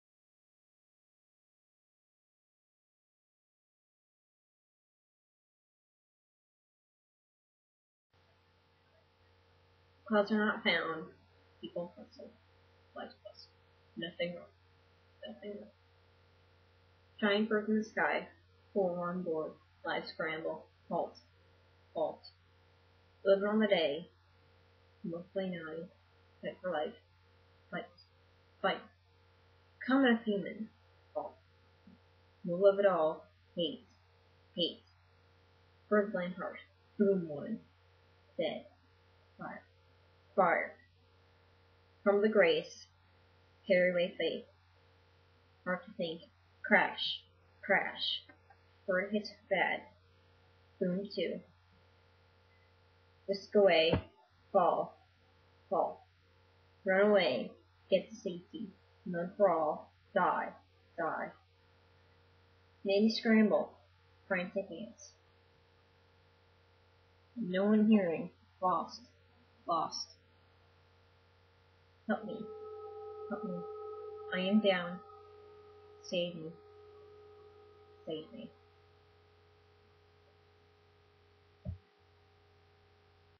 Twin Tower Massacre Save me Save me
Clouds are not found.
People hustle.
Lives bustle.
Nothing wrong.
Nothing wrong.

Giant birds in the sky.
Horror onboard.
Lives scramble.
Hault.
Hault.

Eleven on the day.
Monthly nine.
Fight for life.
Fight.
Fight.

Common of human.
Fault of mind.
No love at all.
Hate.
Hate.

Bird land harsh.
Boom one.
Dead.
Fire.
Fire.

Crumble the grace.
Carry fate away.
Hard to think.
Crash.
Crash.

Bird hits bad.
Boom two.
Whisk away.
Fall.
Fall.

Run away.
Get to safety.
None for all.
Die.
Die.

Many scramble.
Frantic ants.
No one hearing.
Lost.
Lost.

Help me...
Help me...
I am down...
Save me.
Save me.